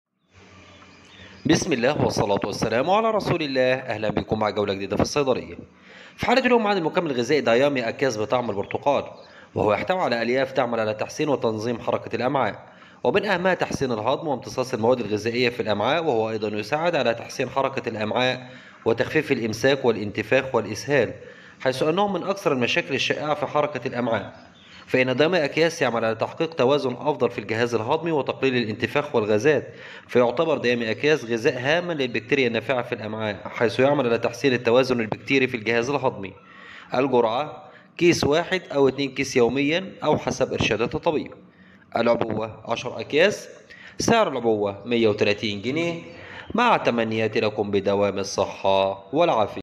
تحسين حركة الامعاء والهضم وامتصاص الغذاء وتخفيف الانتفاخ والامساك والاسهال ودعم البكتريا النافعة
اسم الصنف
ديامي اكياس بطعم البرتقال
DIAMI SACHETS
ويحتوي علي

إينولين (ألياف نقية %100)